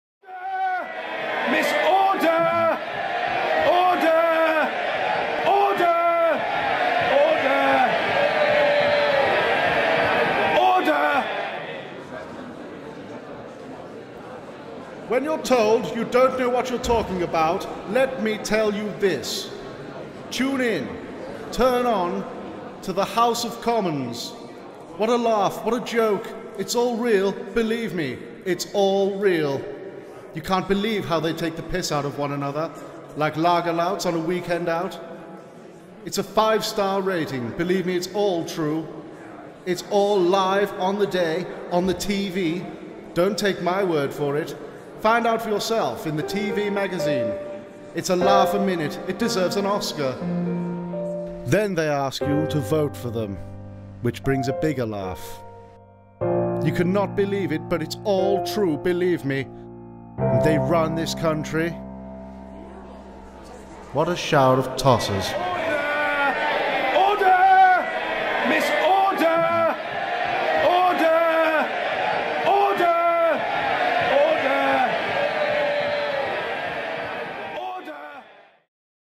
Tom Brookes House Of Commons
Spoken word poem written by Tom Brookes, regarding the state of UK politics.

Recorded and arranged @ Noise Recording